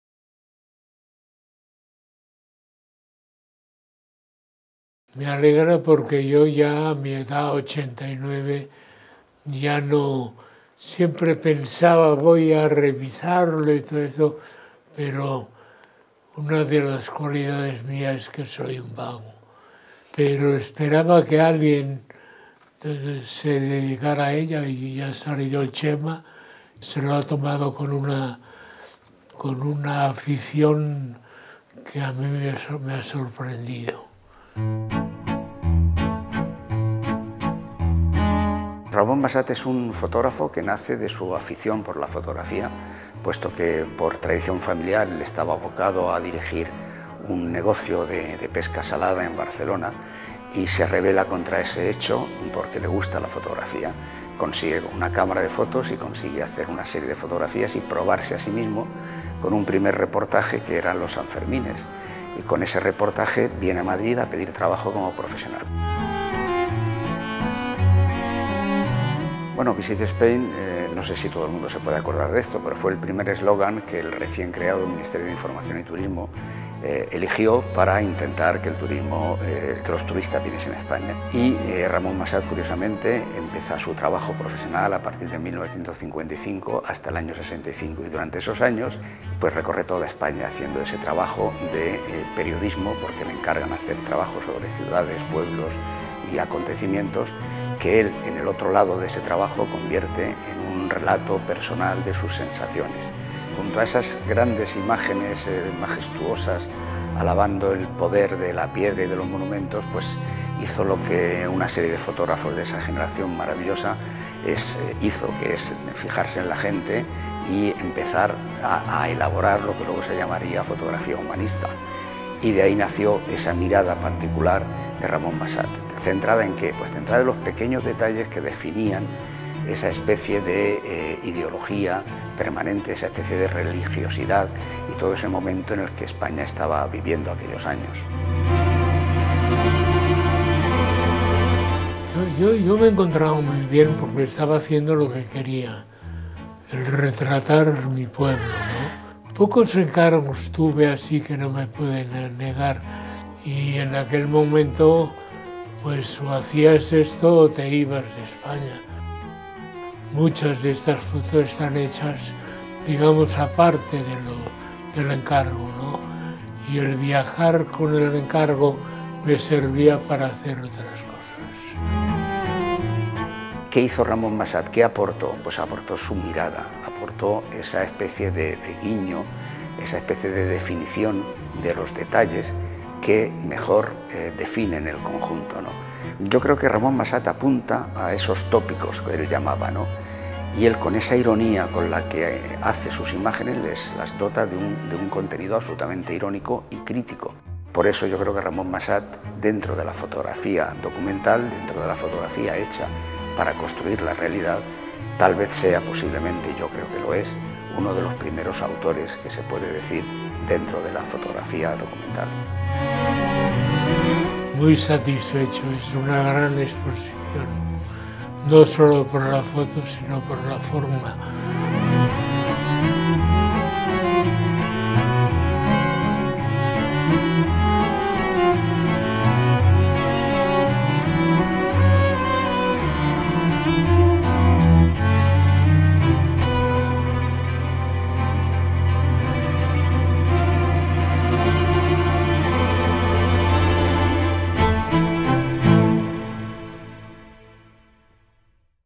Ramón Masats. Visit Spain
Entre 1955 y 1965 Ramón Masats recorre la geografía de España con su cámara y una obsesión en su cabeza: retratar los tópicos con los que la cultura oficial, bendecía los valores patrios. Iniciaba entonces su carrera como colaborador de varias publicaciones, y este viaje le permitió el desarrollo de un trabajo innovador que revolucionó el triste panorama de la fotografía oficial, todavía enredada en su dependencia de los cánones estéticos que el orden clásico imponía a la nueva disciplina.
En el tiempo, el trabajo de Masats coincide con el fin de la autarquía en la dictadura franquista y con la apertura diseñada por el gobierno con la creación del Ministerio de Información y Turismo (1951), y el Plan Nacional de Turismo (1953). Visit Spain fue ese primer mensaje que utilizó la propaganda oficial.
En este escenario Masats emprendió su particular recorrido centrado en los ritos castizos y folclóricos que describe como tópicos. Su relato dibuja una imagen de país atrapado en la pobreza material, laminado en lo social y acérrimo en su atadura espiritual. El poderoso grafismo de sus fotografías y la particular ironía de su mirada significaron la nueva impronta de la fotografía documental, en la que la personalidad del fotógrafo construye una sugerencia más allá de la mera realidad óptica de la imagen fotográfica, y su interpretación final queda en manos solo del espectador. Sugerencias fotográficas que han colonizado nuestra memoria.